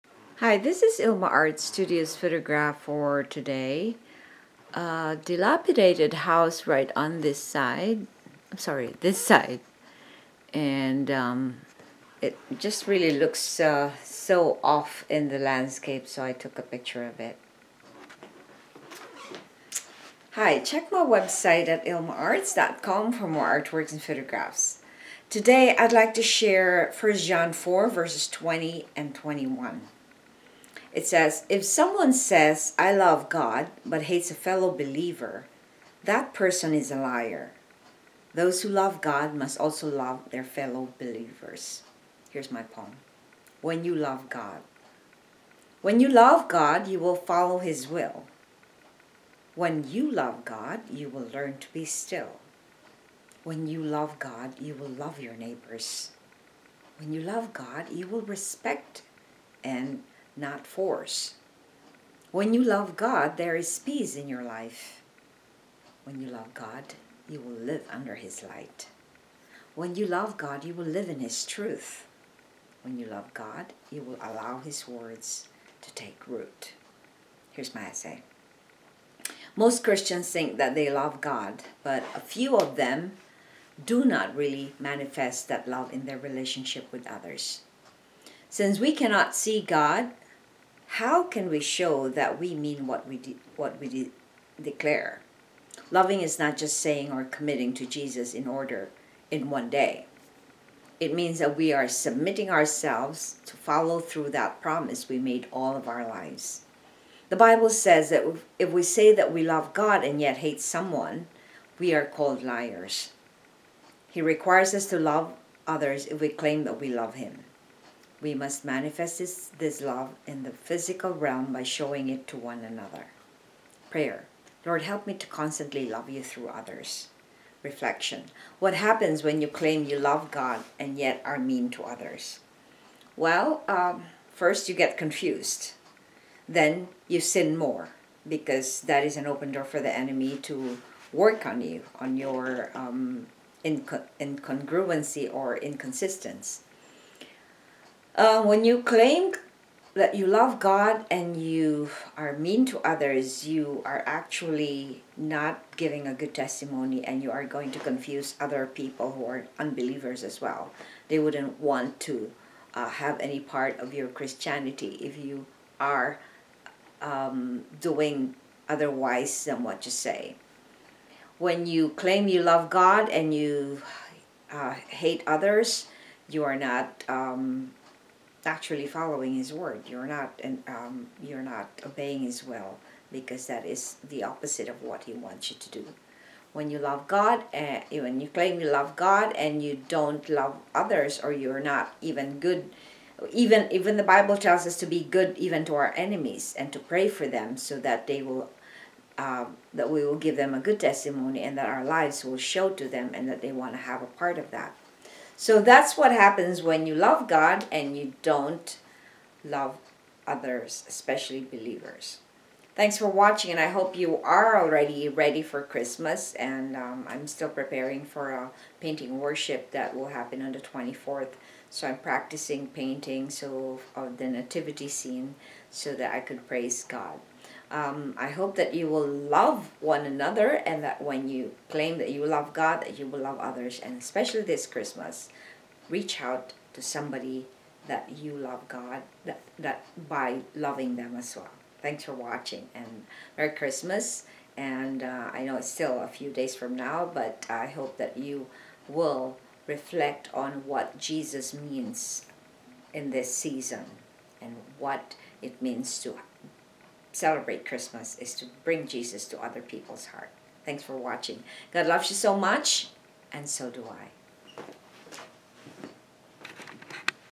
WHEN YOU LOVE GOD poem devotional by ILMA
December 20________________________________________
If someone says, “I love God,” but hates a fellow believer, that person is a liar. Those who love God must also love their fellow believers. – 1 John 4:20&21(NLT)
WHEN YOU LOVE GOD
When you love God, you will follow his will
When you love God, you will learn to be still
When you love God, you will love your neighbors
When you love God, you will respect and not force
When you love God, there is peace in your life
When you love God, you will live under his light
When you love God, you will live in his truth
When you love God, you will allow his words to take root.
Most Christians think that they love God, but a few of them do not really manifest that love in their relationships with others. Since we cannot see God, how can we show that we mean what we declare? Loving is not just saying or committing to Jesus in one day. It means that we are submitting ourselves to follow through that promise we made all of our lives.
The bible says that if we say that we love God and yet hate someone, we are called liars. He requires us to love others if we claim that we love him. We must manifest this love in the physical realm by showing it to one another.
PRAYER
Lord, help me to constantly love you through others.
REFLECTION
What happens when you claim you love God and yet are mean to others?
For more testimonials about God’s word, please go to YouTube and type ILMA’s poem devotional or go posts.